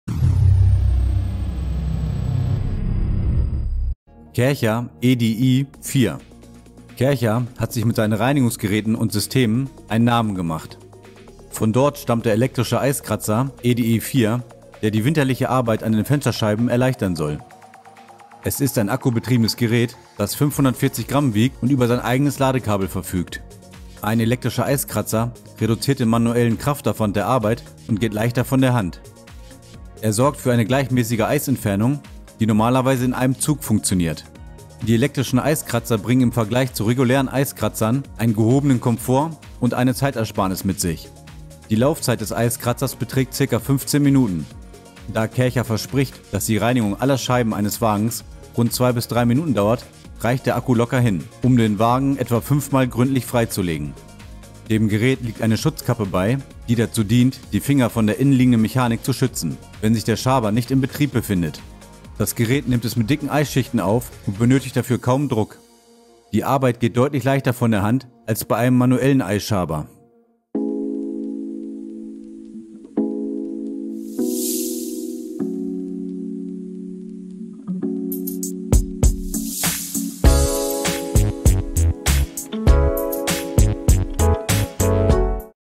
Elektrischer Eiskratzer TEST - Lohnt sich der Kärcher EDI 4?
Der beste elektrische Eiskratzer  ►Welchen elektrischen Eiskratzer kaufen ?
►Kärcher EDI 4

Liebe Grüße
Dein Vergleichssieger-Team